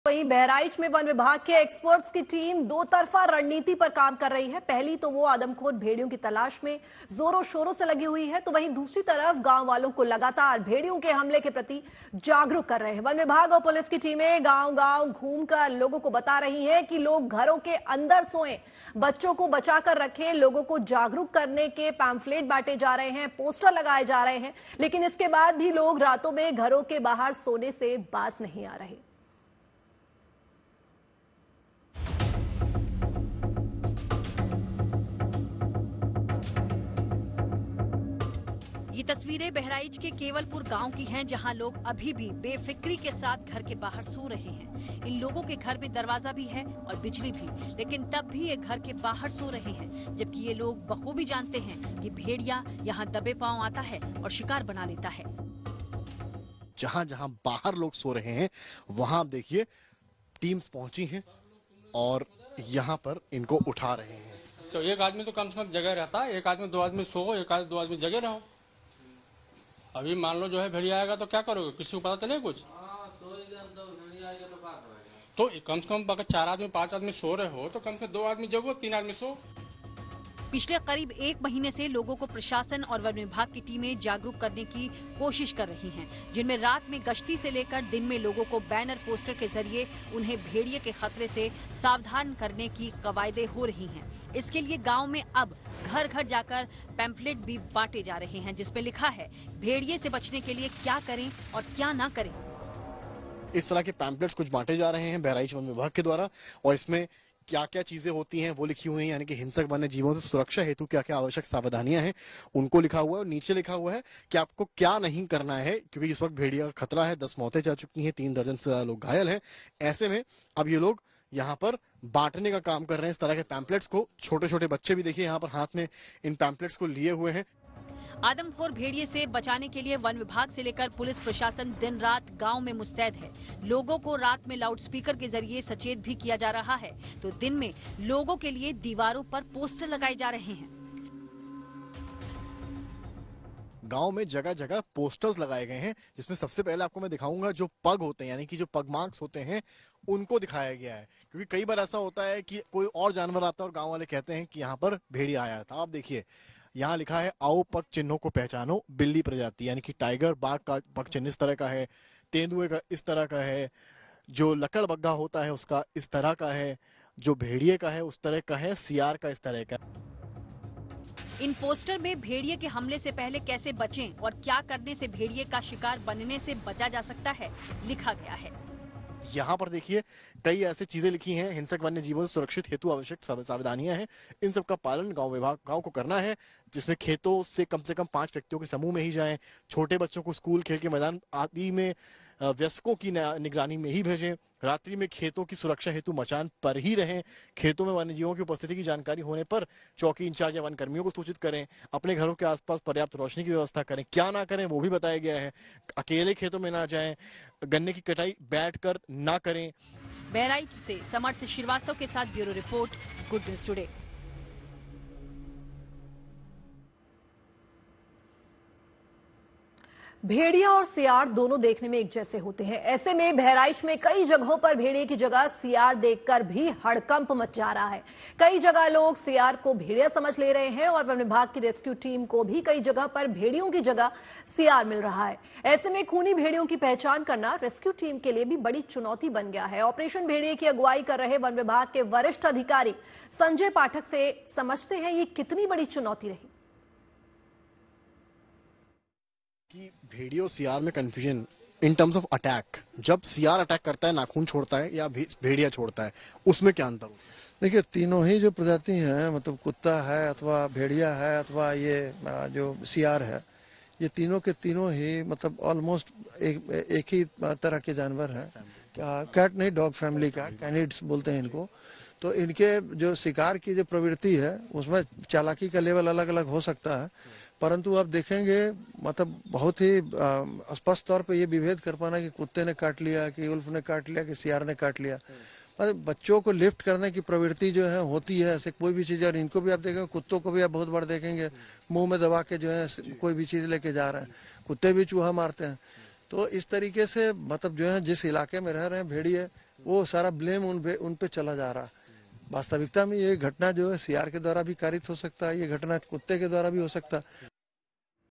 Bahraich में लंगड़ा भेड़िया अभी भी घूम रहा आजाद, ग्रामीणों को दी जा रही अलर्ट रहने की सलाह | GNT TV
Bahraich Wolf Attack: बहराइच के केवलपुर गांव में लोग अभी भी बेफिक्री के साथ घर के बाहर सो रहे हैं. इन लोगों के घर में दरवाजा भी है और बिजली भी, लेकिन तब भी ये घर के बाहर सो रहे हैं. जबकि ये लोग जानते हैं कि भेड़िया यहां दबे पांव आता है और शिकार बना लेता है. वन विभाग और पुलिस की टीमें गांव गांव घूम कर लोगों को बता रही हैं कि लोग घरों के अंदर सोएं. बच्चों को बचा कर रखें. लोगों को जागरुक करने के पंफलेट बांटे जा रहे हैं. पोस्टर लगाए गये हैं.